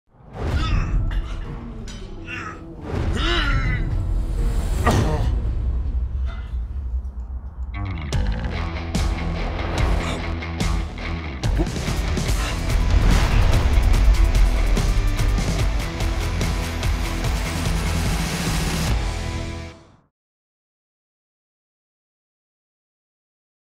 HUAWEI Mate40 Pro l عيونك على الشاشة
احصل على التشجيع الذي تحتاجه وقتما تشاء، من خلال ميزة عيونكم على شاشة سلسلة
HUAWEIMate40 Pro